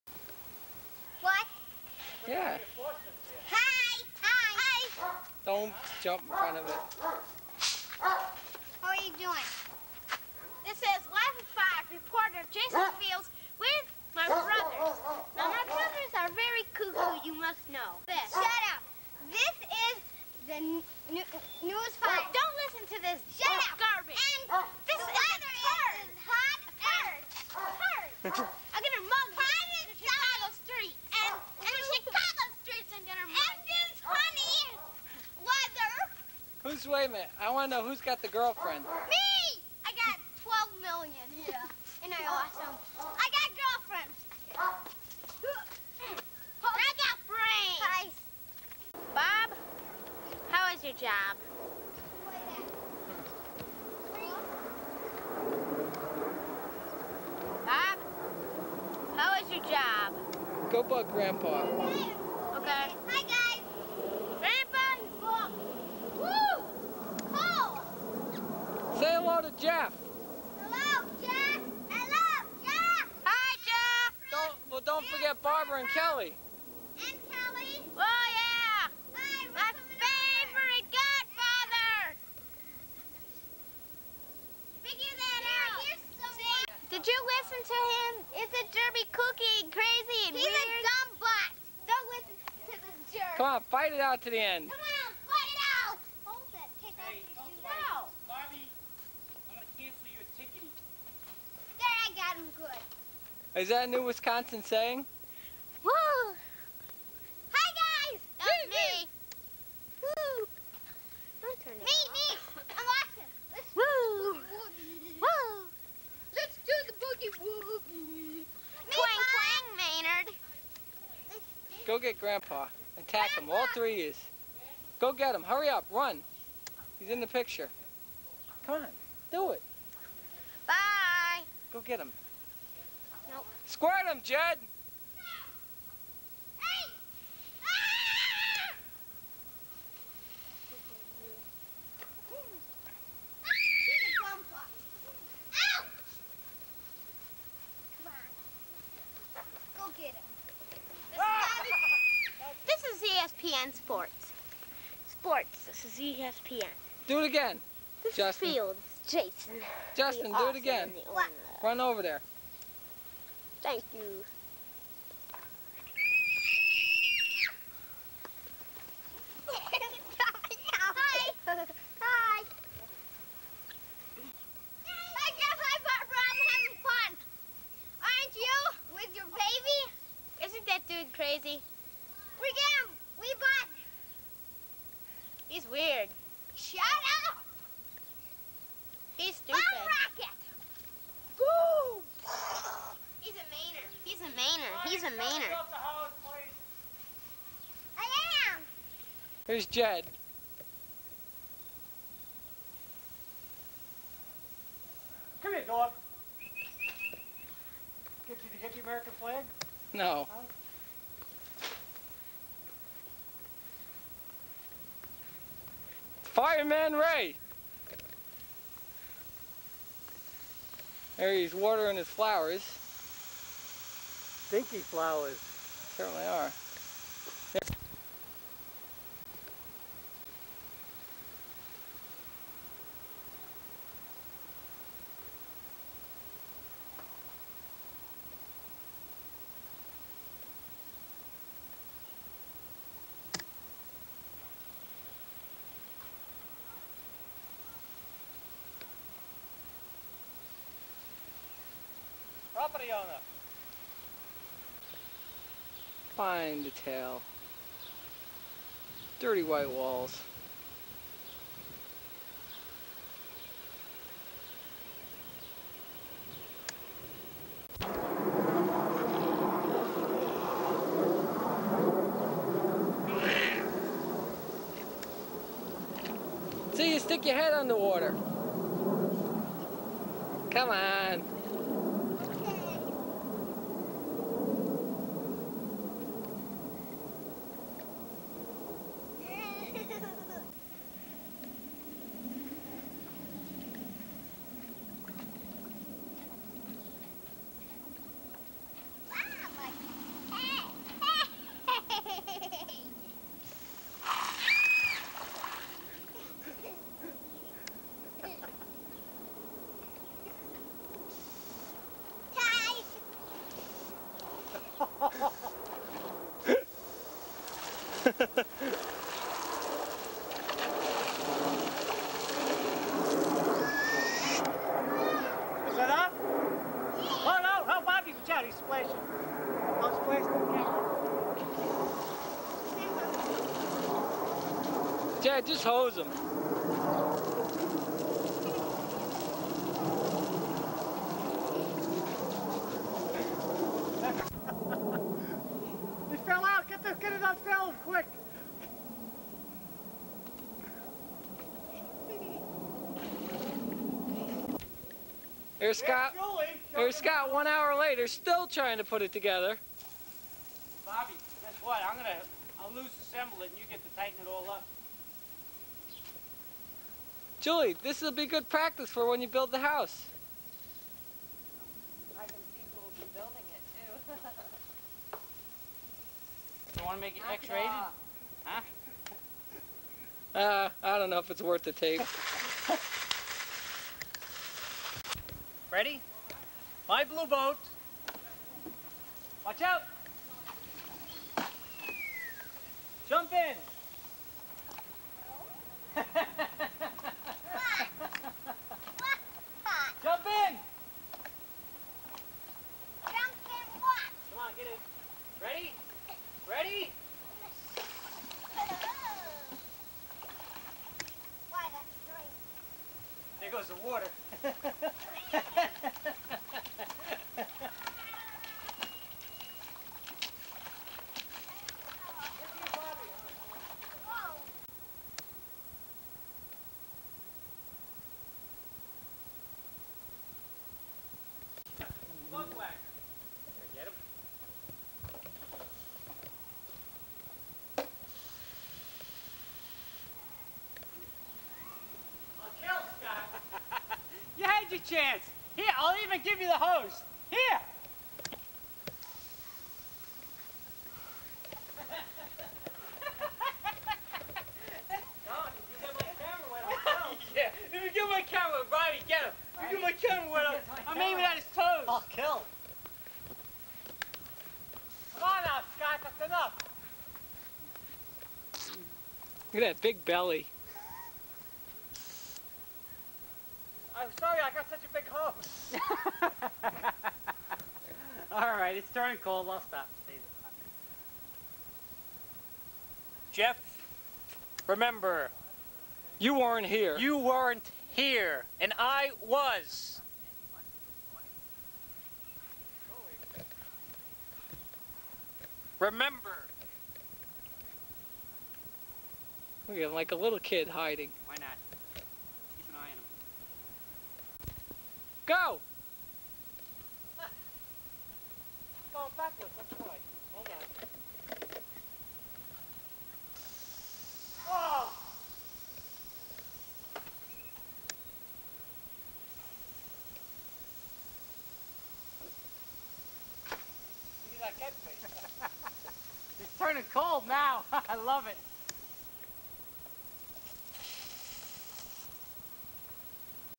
July 4th 1985.mp4
The three J's come visit Western Springs on the Fourth of July, 1985. Let the madness begin...